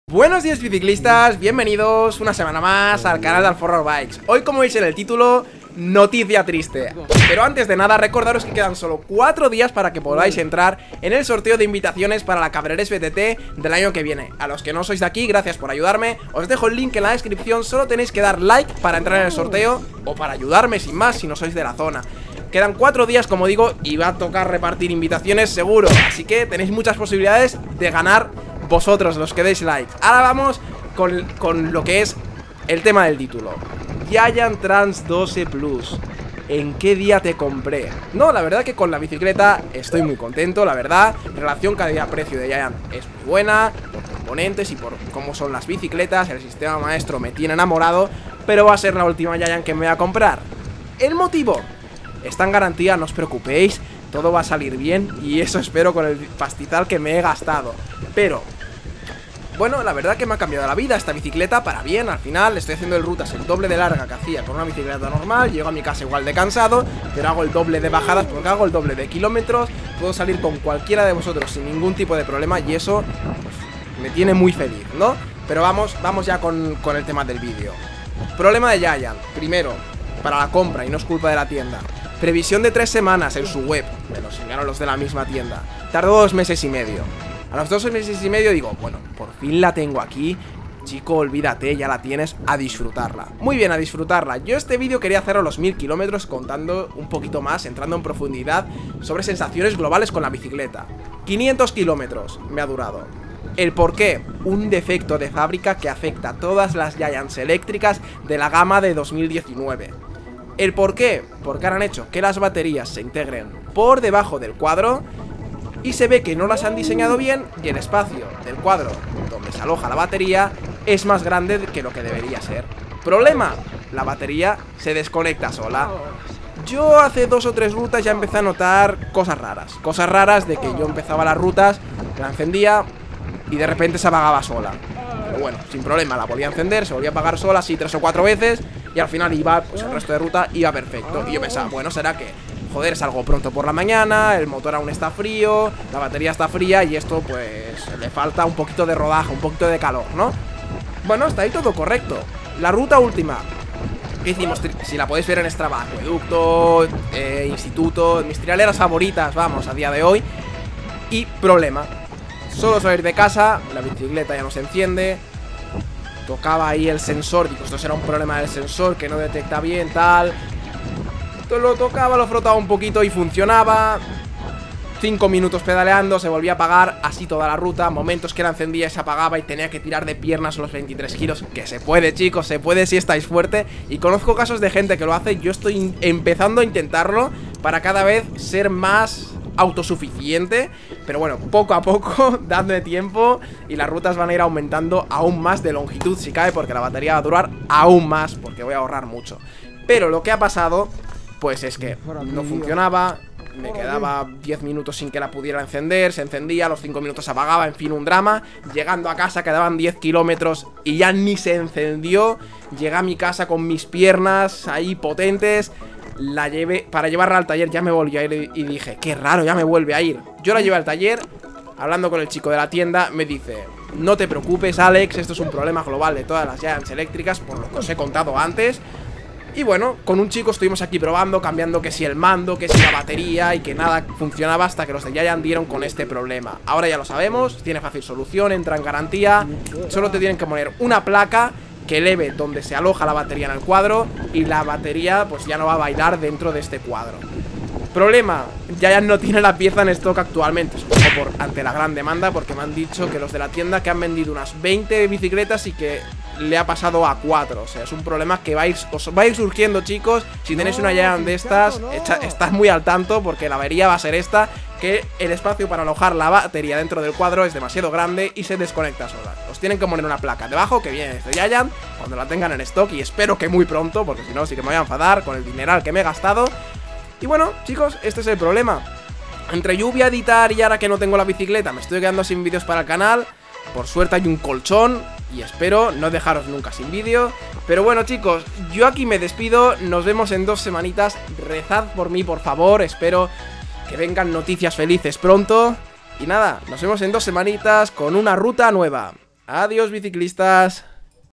PROBLEMA de FÁBRICA en GIANT TRANCE 2 E+
En el vídeo de hoy, os cuento un drama más de mi vida ciclista. Lo hago ya por si sirve de algo para alguien que ya tenga o se plantee comprar una de esta marca!